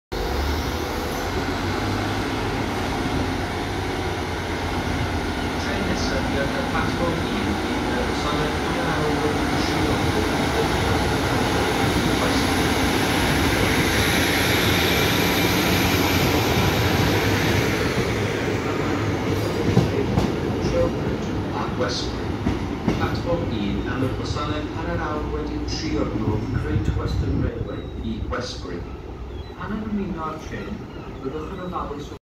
TfW Class 150 Departing Cardiff Central - 150217 | 10/8/23
Transport For Wales' 150217 departing Cardiff Central doing the 14:56 Barry Island to Merthyr Tydfil service.